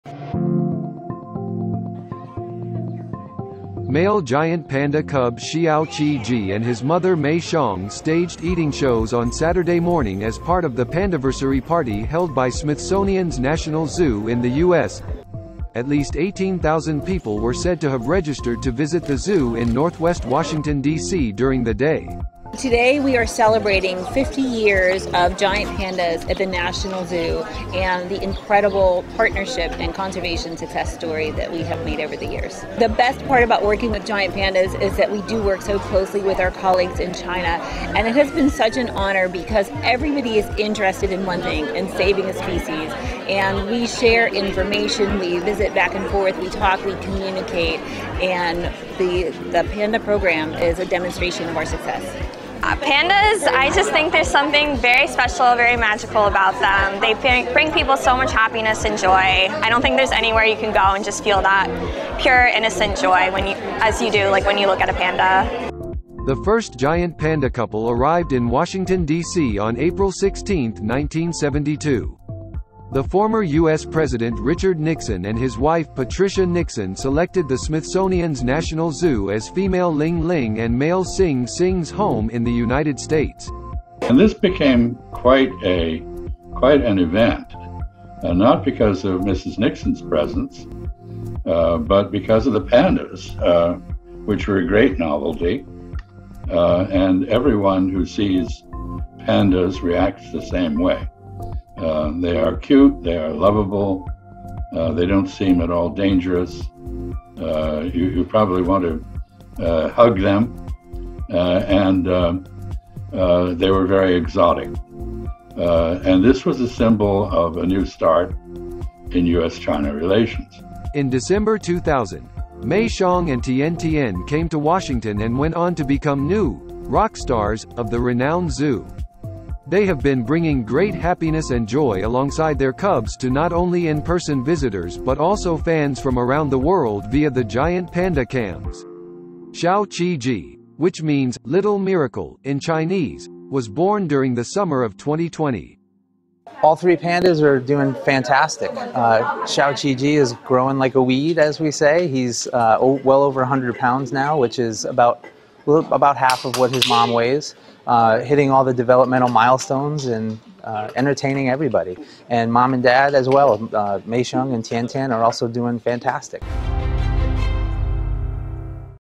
GLOBALink | U.S. zoo marks China-U.S. collaboration on giant panda conservation
"We are celebrating 50 years of giant pandas... and the incredible partnership and conservation success story that we have made over the years," said the director of the Smithsonian's National Zoo on China-U.S. collaboration on giant panda conservation.